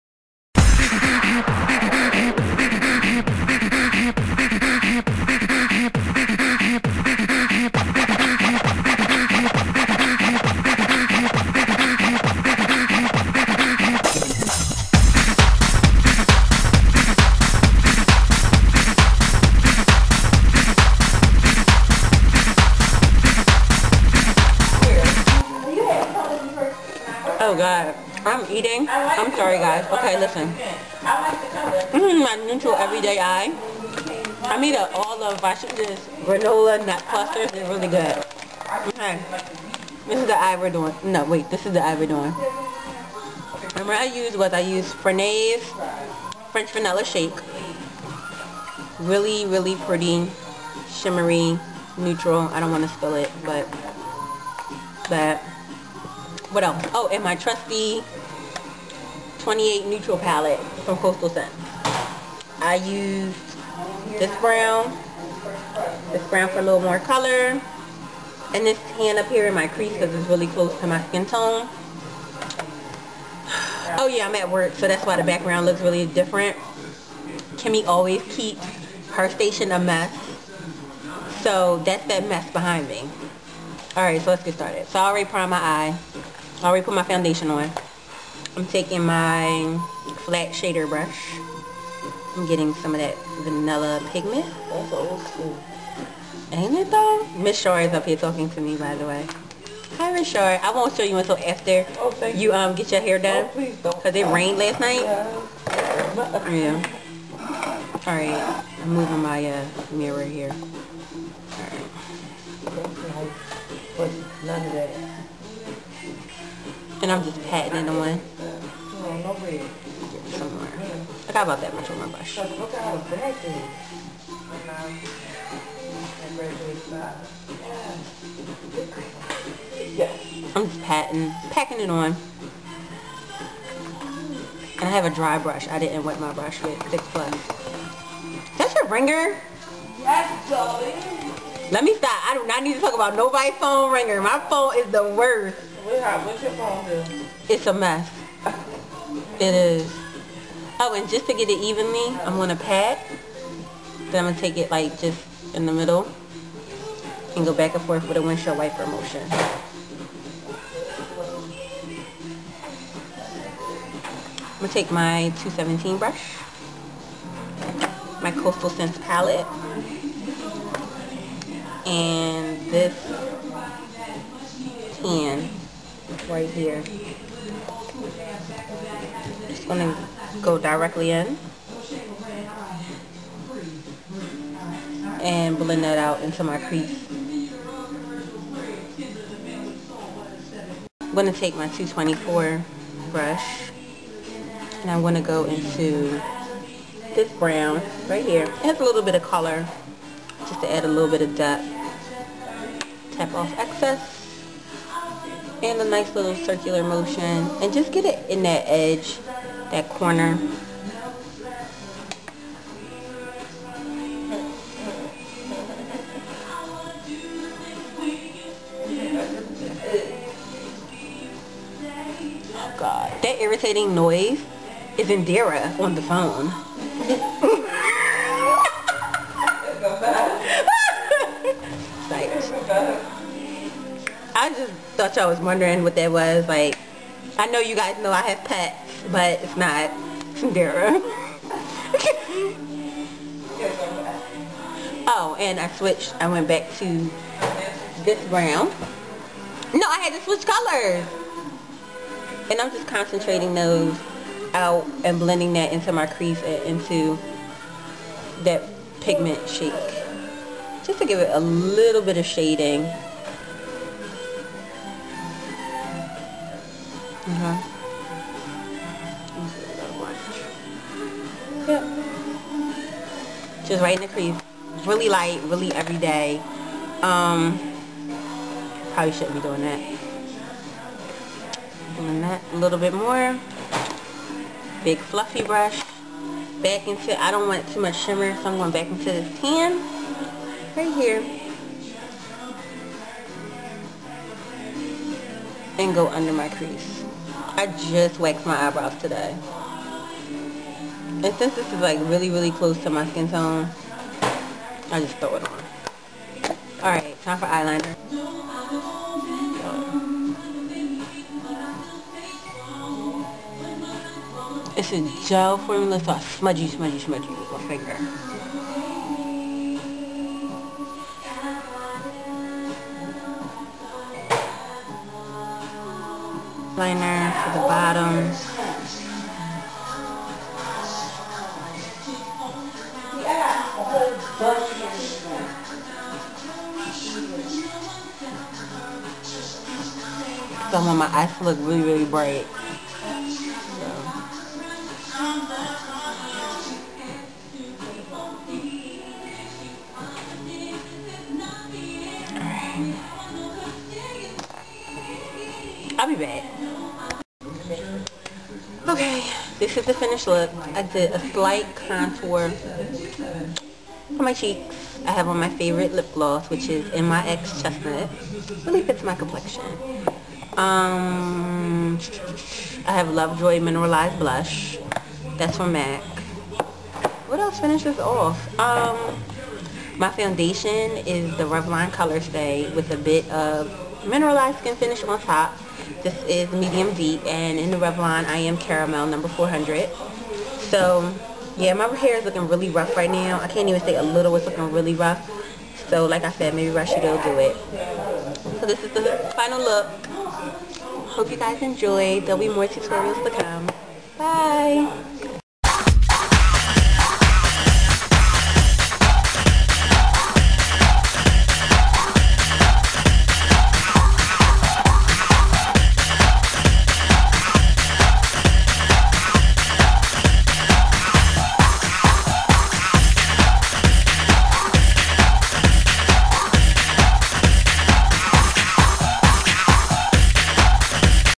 Makeup Tutorial: My Everyday Vixen
Hey everyone this is my everyday look when I go to work!!!  I know I was being a little silly at work but it's ok everyone should have a good time!! :)